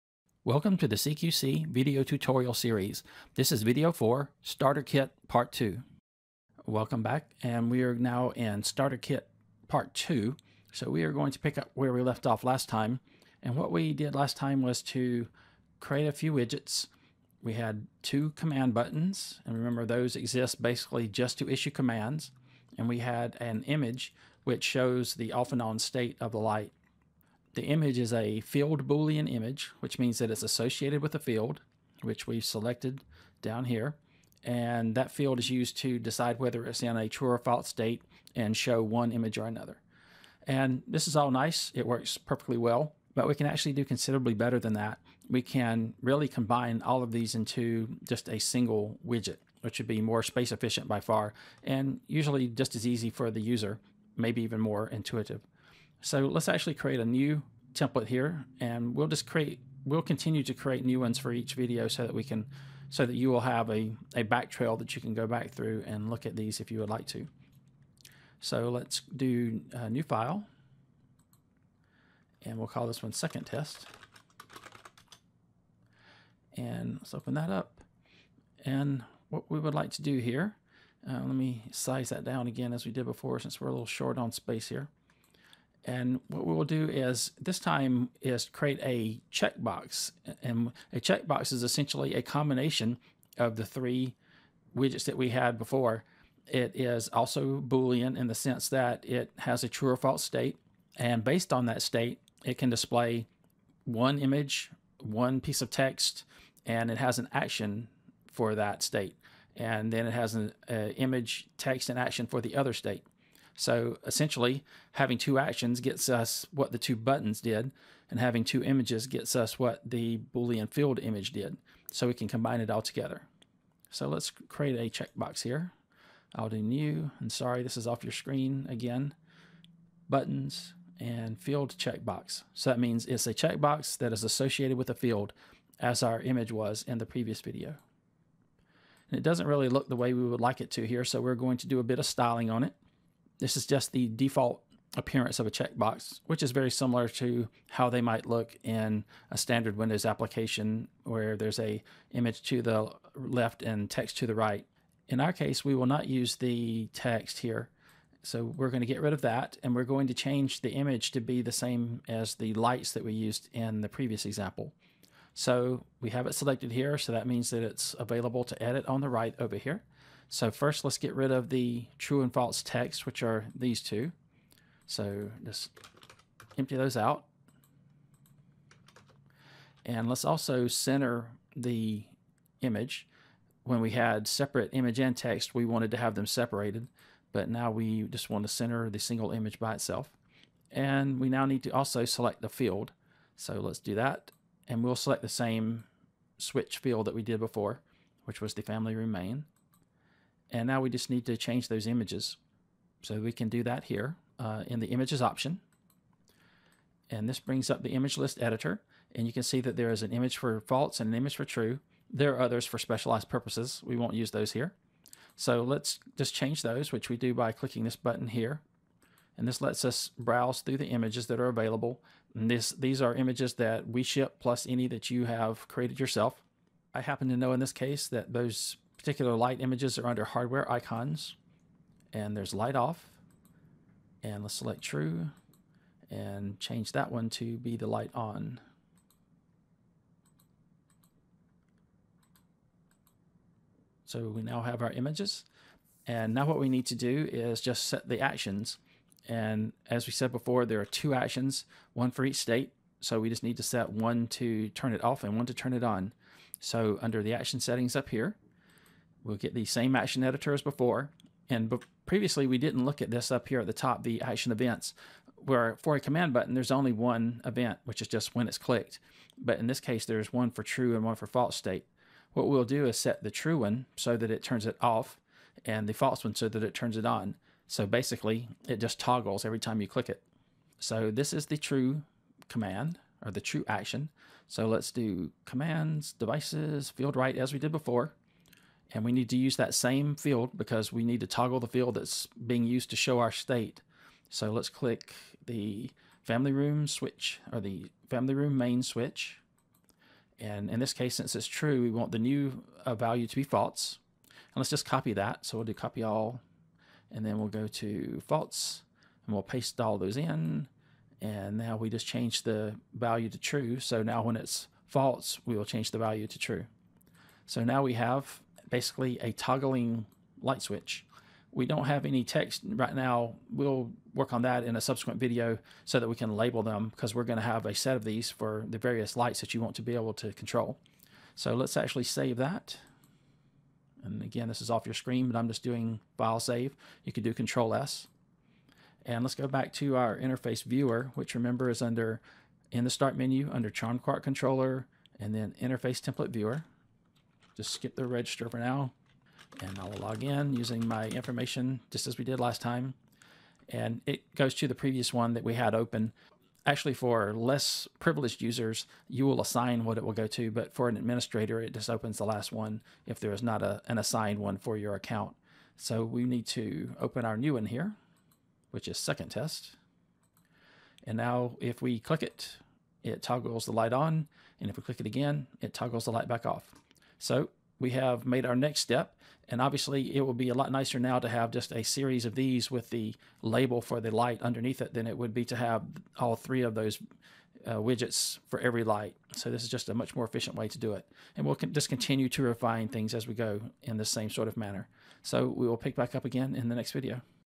CQC 5.0 - Starter Kit 2
This is the 4th video in the tutorial series, and picks up where the previous one left off, and introduces a new type of interface widget.